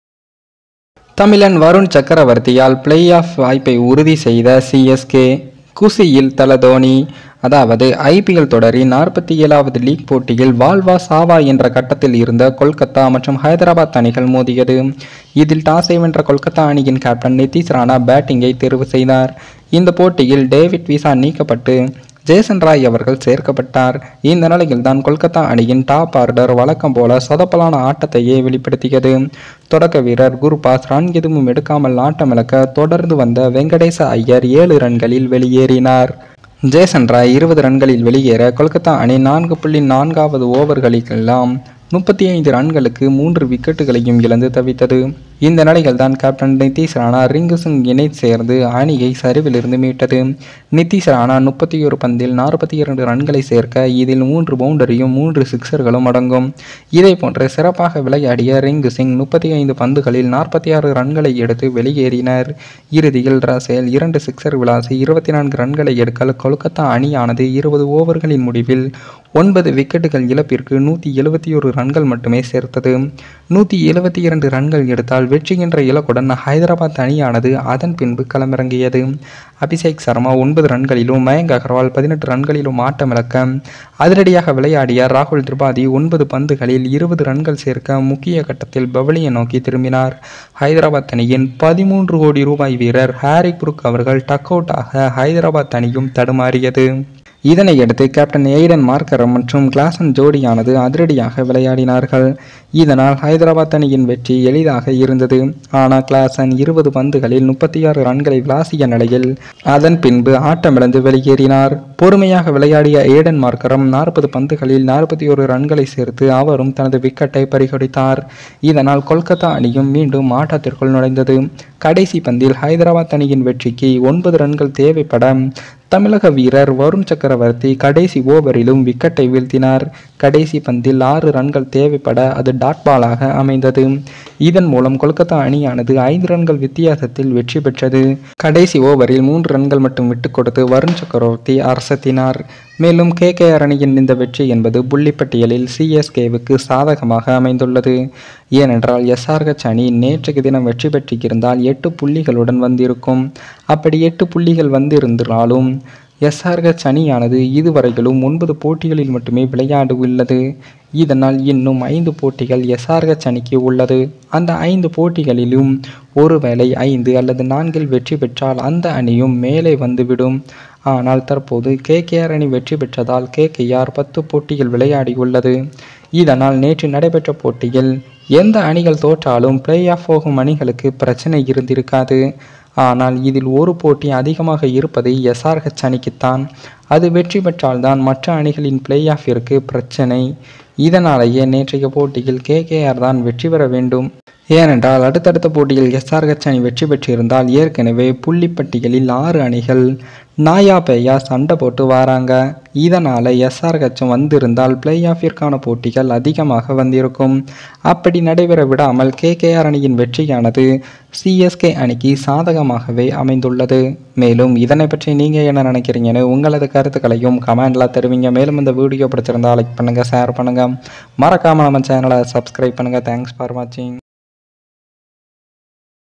தமிழன் வருண் செய்த மேஜிக்! CSK Play Off வாய்ப்பு உறுதி! குஷியில் தல தோனி இத கவனித்தீர்களா! KKR v SRH
Support frd's 🙏